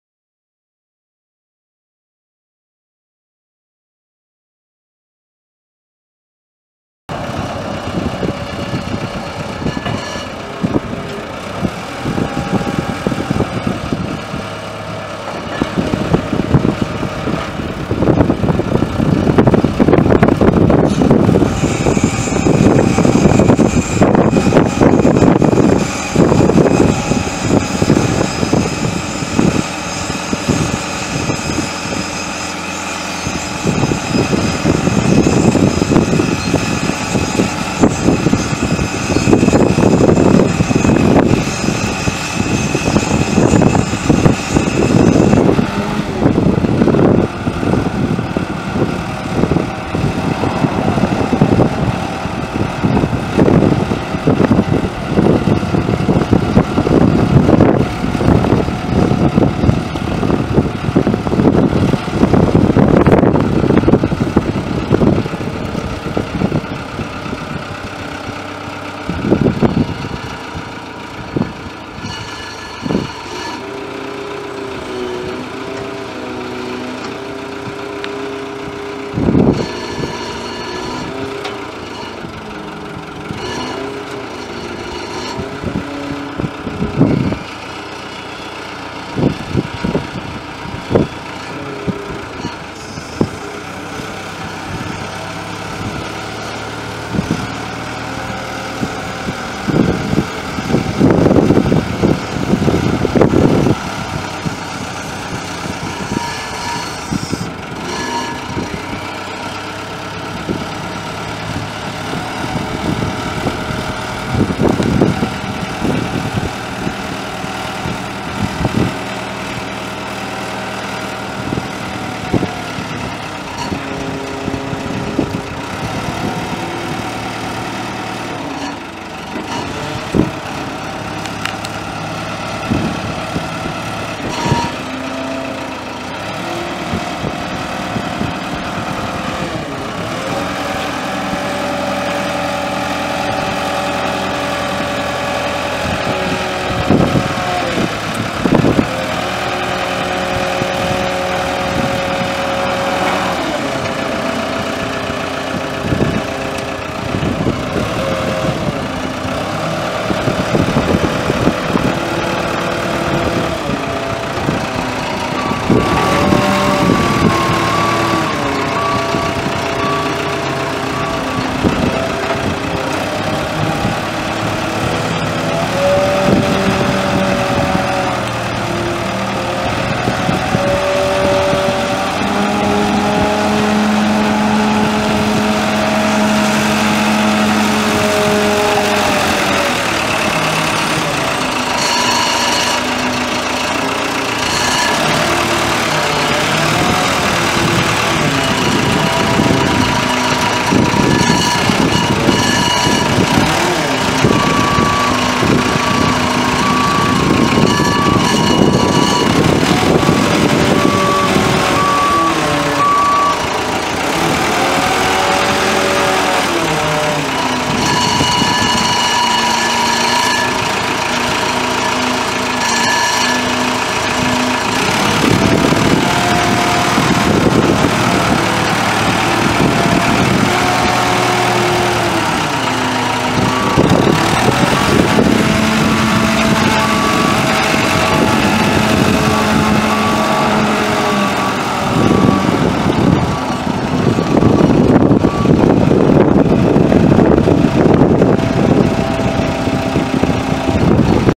1999 SkyTrack 10542 telehandler for sale | sold at auction May 29, 2014
1999 SkyTrack 10542 telehandler
7,258 hours on meter
Cummins B3.9 four cylinder diesel engine
116 HP
Serial 45889332
Powershift transmission
4F-4R gears
Four wheel drive
Locking differential
Circle steer
Crab steer
ROPS
Spring suspension seat
Work light
10,000 lbs maximum lift capacity
42&#039; maximum lift height
Four boom sections
48"L x 4.5"W forks
Hydraulic outriggers
Tilt carriage
Rotating carriage
Hydraulic leveling
Quick attach
Michelin 17.5R25 tires
Three tires foam filled
27,300 lbs. base operating weight
Skytrak 10054 graphics
Serial 10740

It was located in Norman, OK, and we sold it to the highest bidder for $36,300.